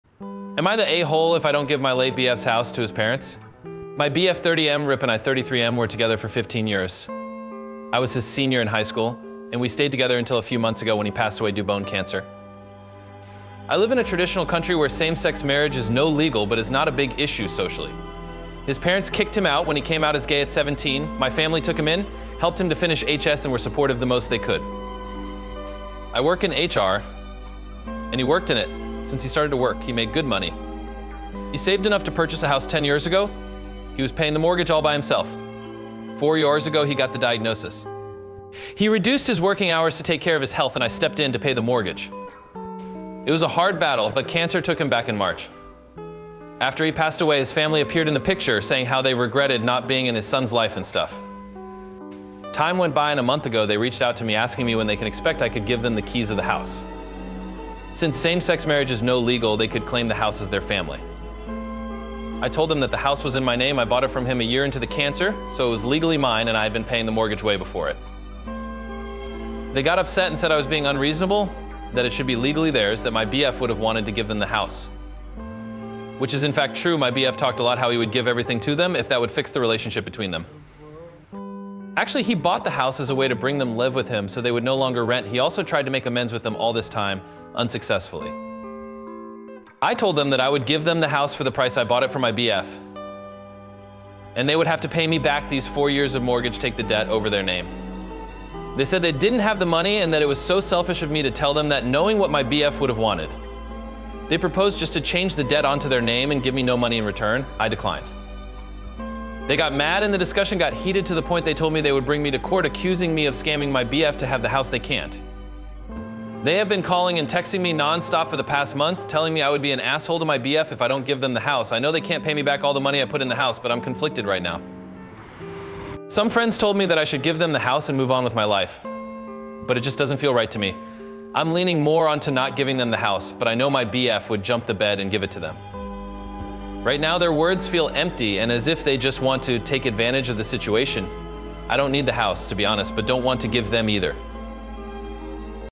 AITA for Refusing to Give My Late Boyfriend's House to His Estranged Parents?
The user, a 33-year-old man, was in a 15-year relationship with his late boyfriend, who passed away from bone cancer. Despite being disowned by his parents at 17 for being gay, the boyfriend managed to purchase a house 10 years ago. The user’s family took him in, and the couple lived together, with the user eventually taking over the mortgage when his boyfriend's health declined. After the boyfriend's death, his estranged parents reappeared and demanded the house, claiming it should be theirs, even though they had been absent from his life for years. The user is torn between his late boyfriend’s wishes to mend ties with his family and his own feelings of being taken advantage of, especially since the house is legally in his name. The parents have been pressuring him, and he feels conflicted about whether he should give them the house or not.